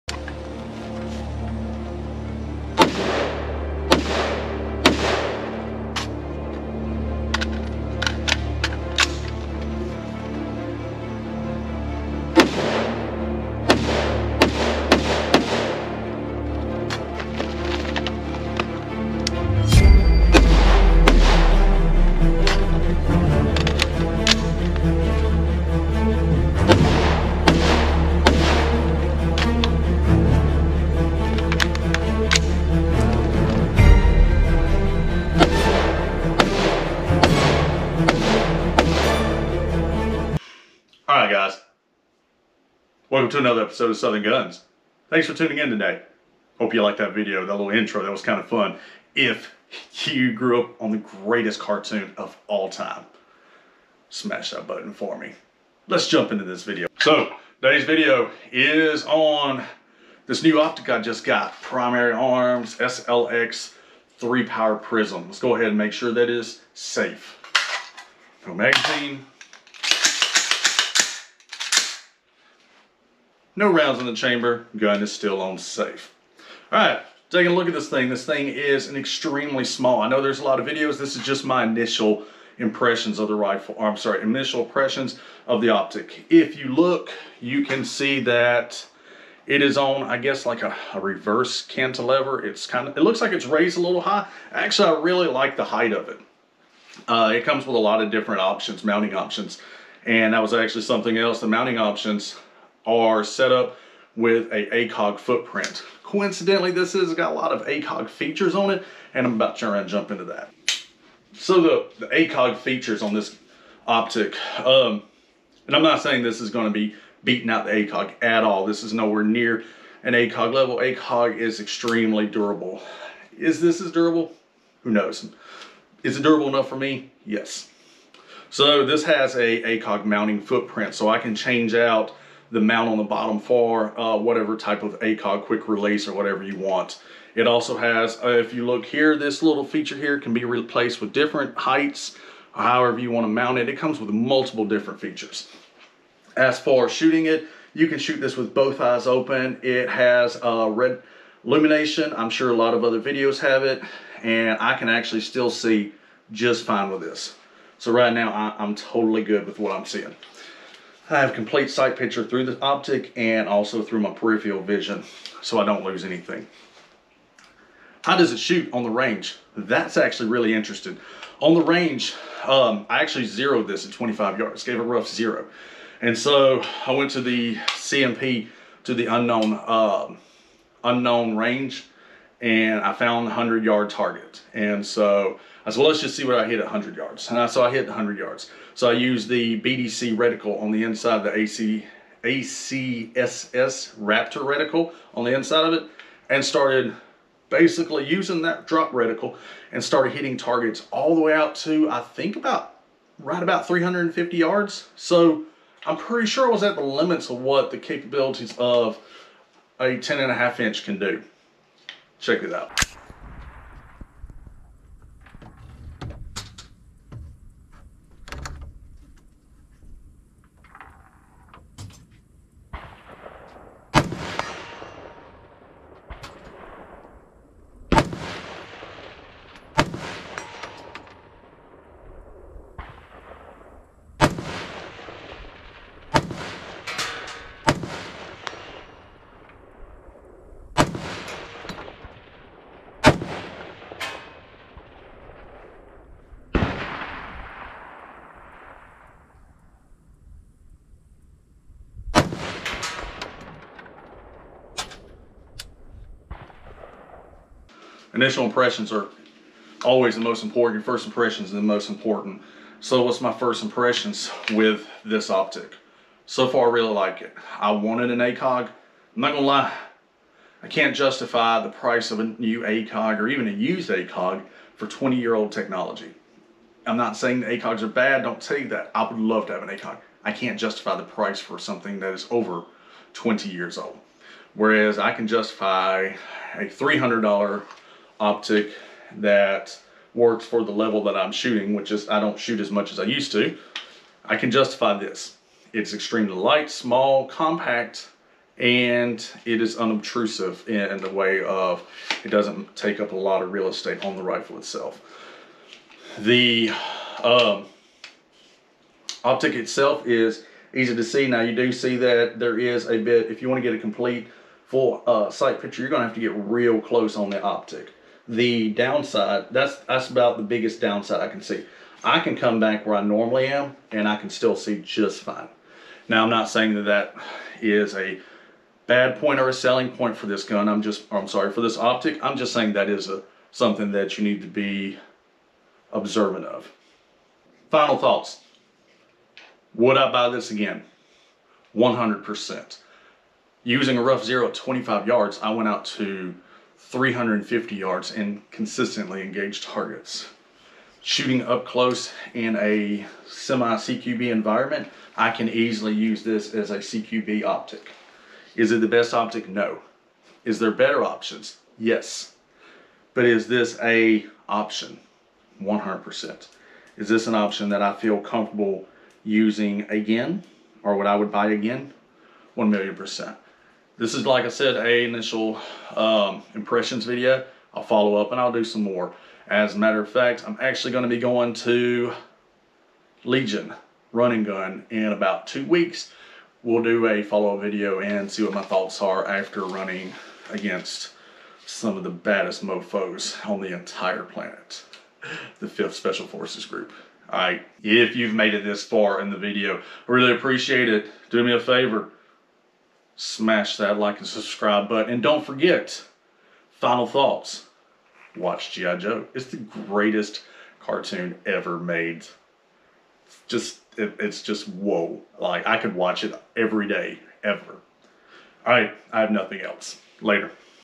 Primary Arms SLX 3x prism | Alternative to the ACOG?
Initial impressions of the Primary Arms 3x Micro prism.  Is it an viable option to the ACOG? First impressions are everything, and so far it's looking good.
Roughly zeroed at 25 yards, and then took it straight out to 300 yards with it on the unknown distance range at the CMP marksmanship park.

Helikon-tex Defender shirt